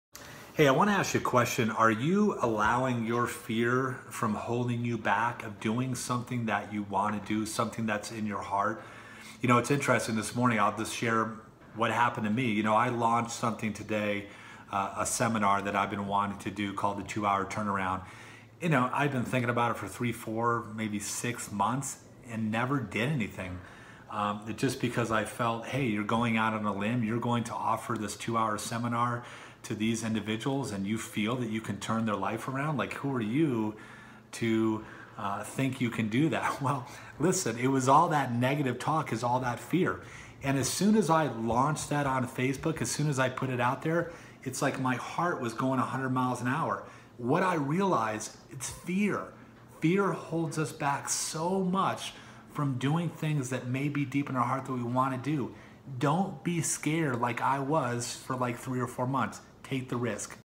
DON'T LET FEAR HOLD YOU BACK / Mindset Minute #56
If you are not careful you can let fear hold you back from acting on a passion that may be in your heart and before you know it that passion may be no more. That is a sad reality for too many people